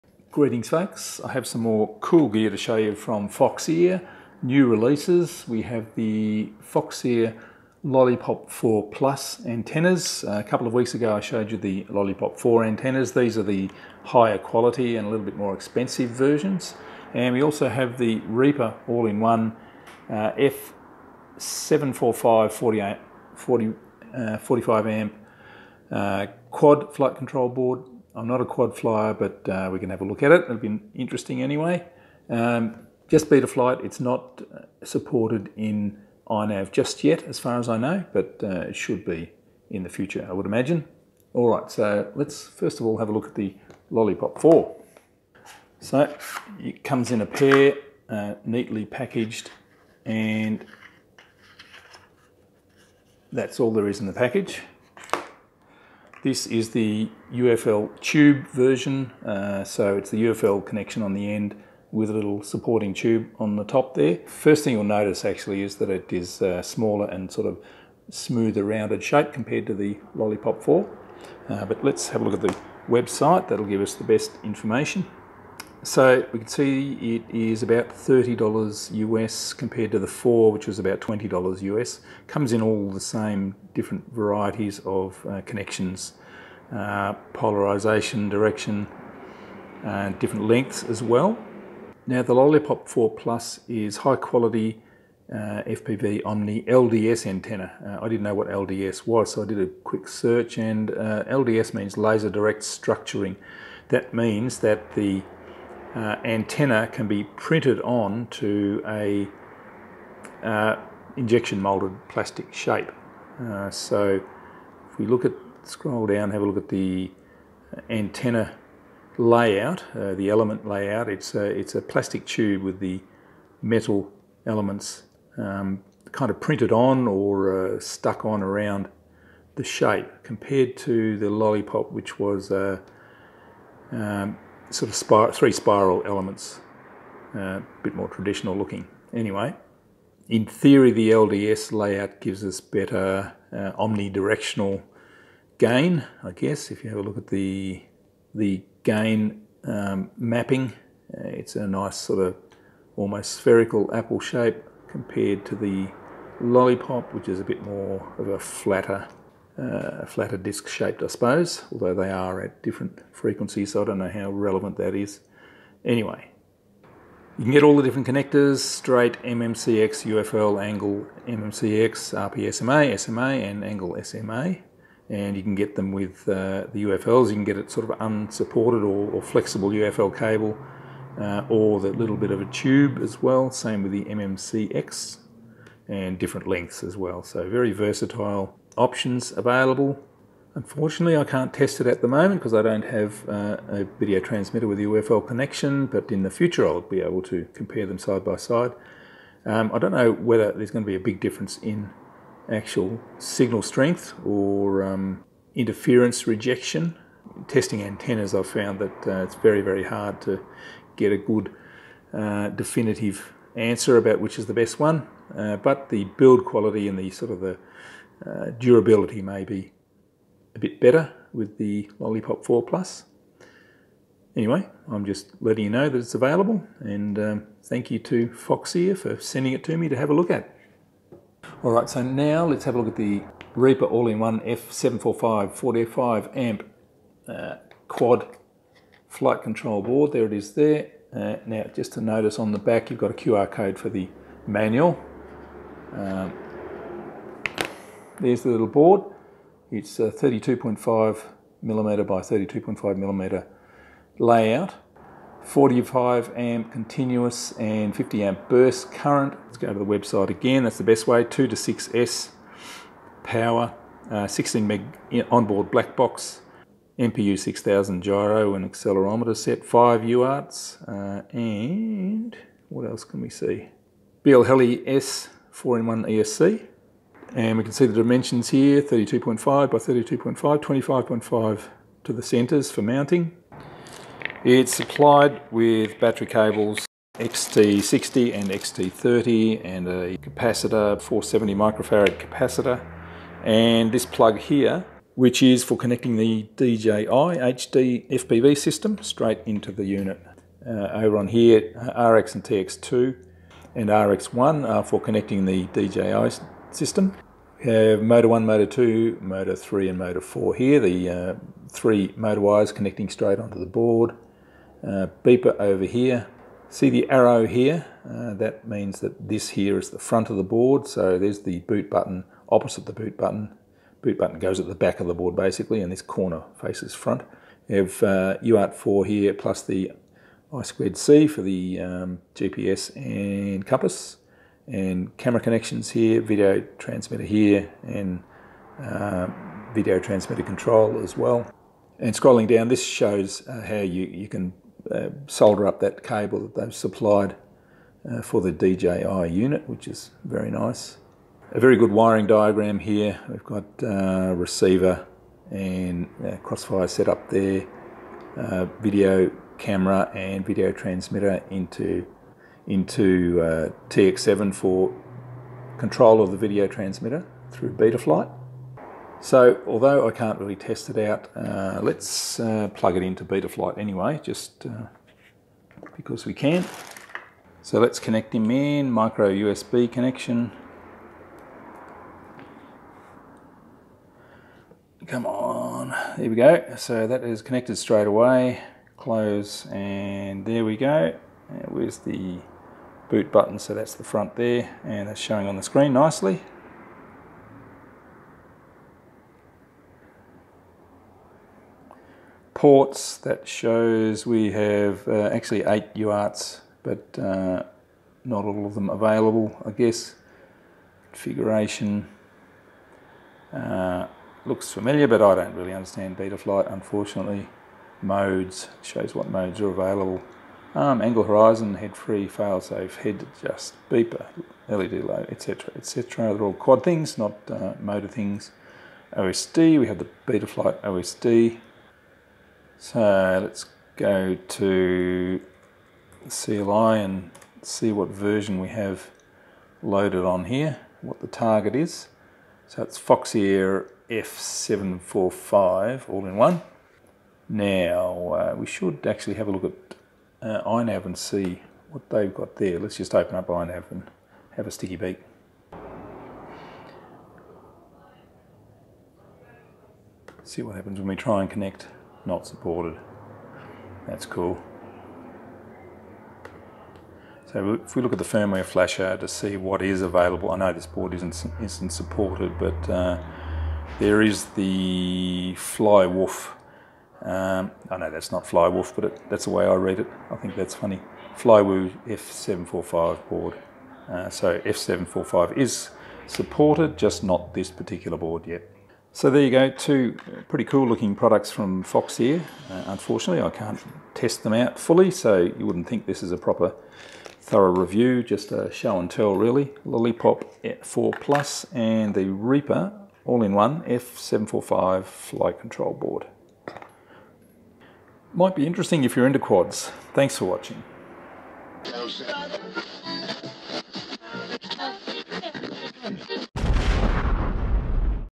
Foxeer Reaper F745 AIO FCB and Lollipop 4+ antennas
Quick show and tell of the new Foxeer Reaper F745 AIO Quad flight board and Lollipop 4+ antennas.

This is a fun hobby for me, not a money making venture.